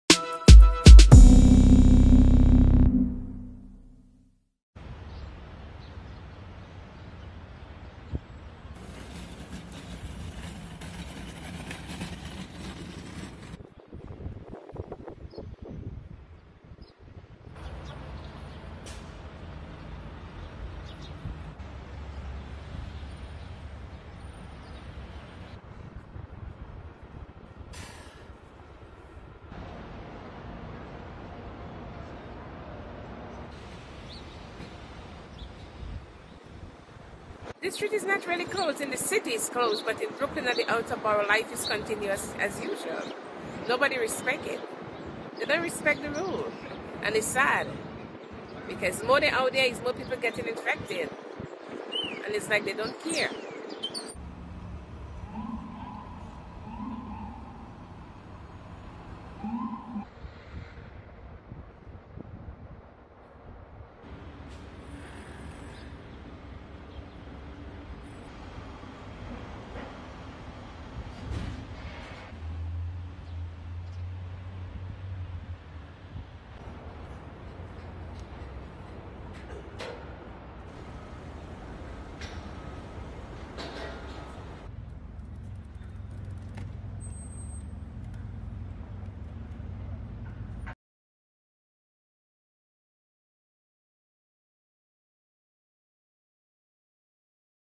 Νέα Υόρκη: Υπό πίεση τα νοσοκομεία
Περισσότερα από 23.000 κρούσματα, 365 νεκροί: η Νέα Υόρκη είναι το επίκεντρο της επιδημίας του κορονοϊού στις ΗΠΑ. Τα νοσοκομεία βρίσκονται υπό πίεση, αλλά καταφέρνουν ακόμη να αντεπεξέλθουν. Πηγή RUPTLY